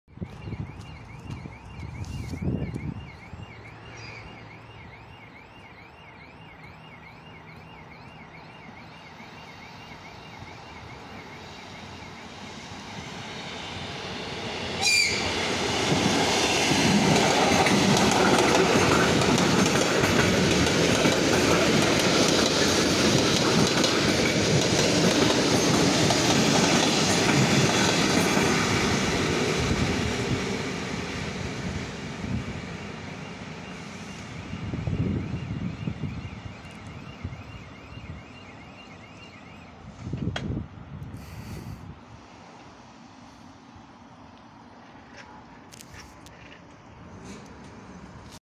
"Опасная кривая" несется Эр9м-551 №6029 Киев-Мироновка переезд Глеваха перегон Боярка-Васильков 1
VID_20200722_193153 эр9м-551 6029 Мироновка глеваха.mp4